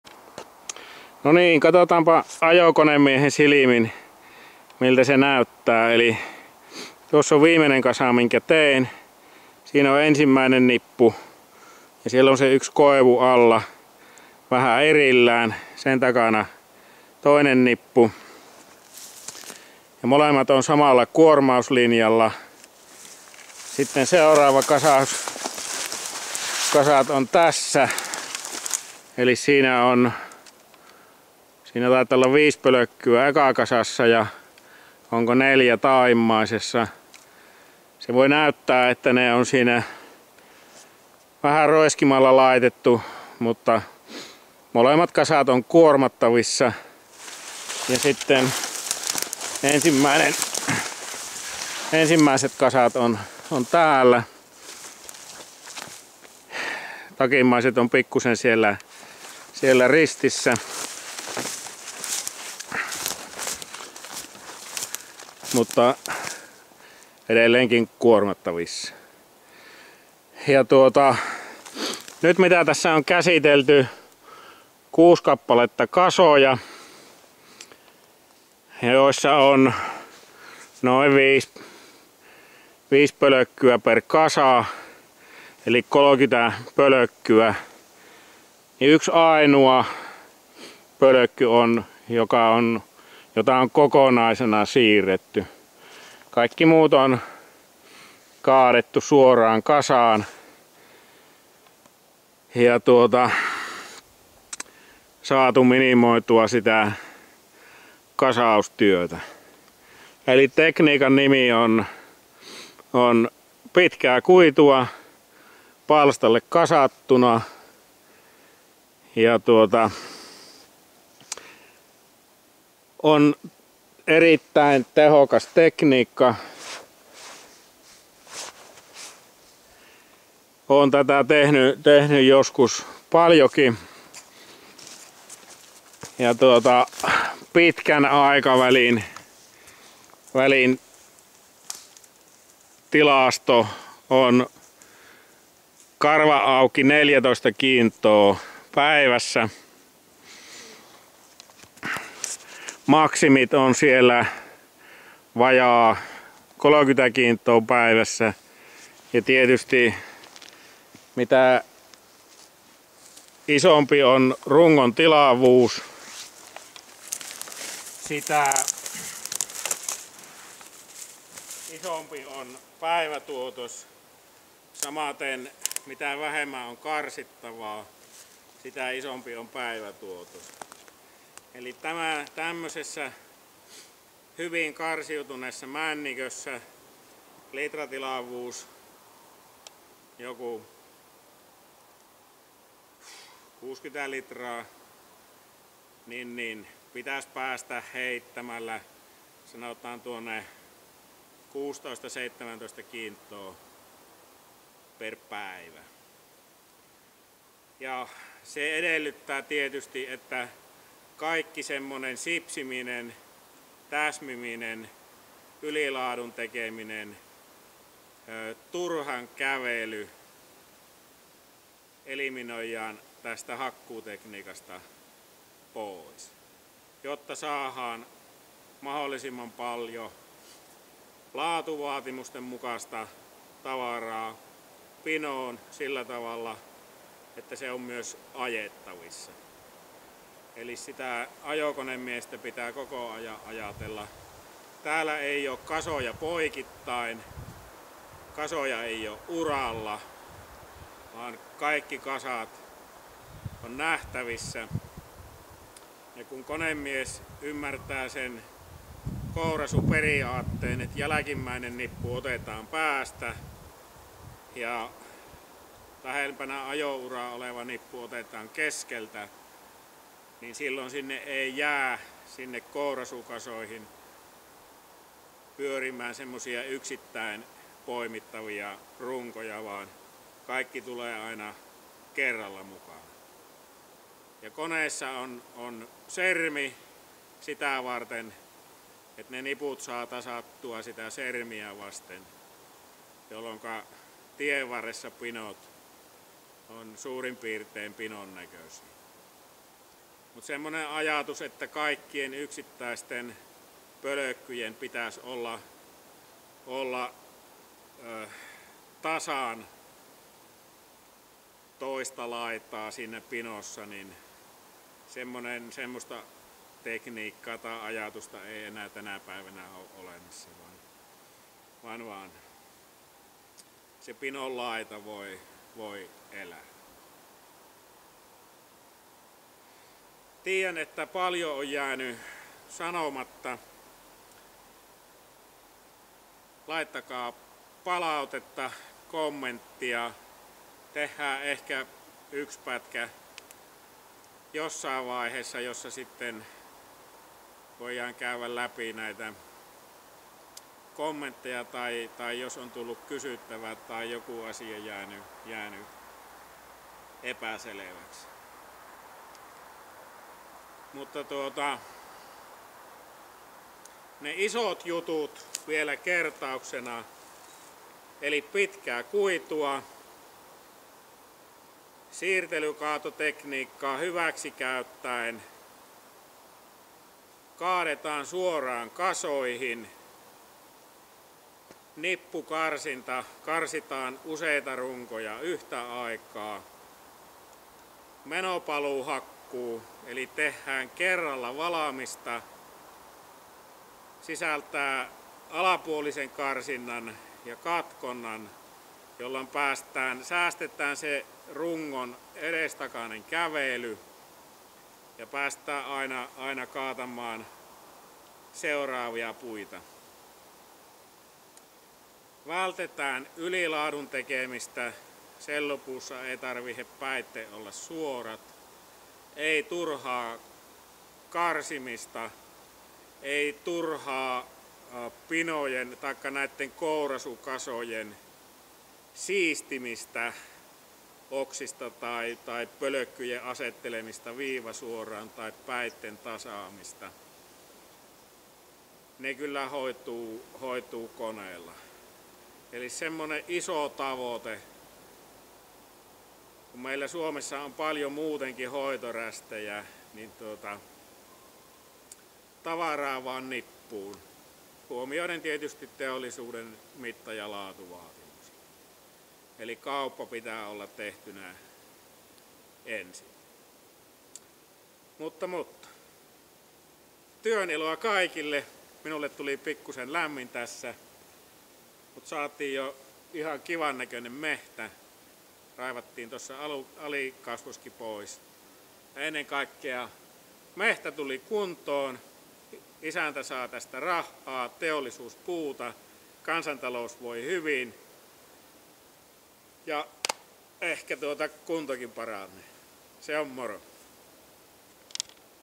Metsolan Metsäkoulu 32, Ensiharvennuksen hakkuutekniikka. Lopputulos ja loppusanat
Työniloa kaikille ja tehkää turvallisesti!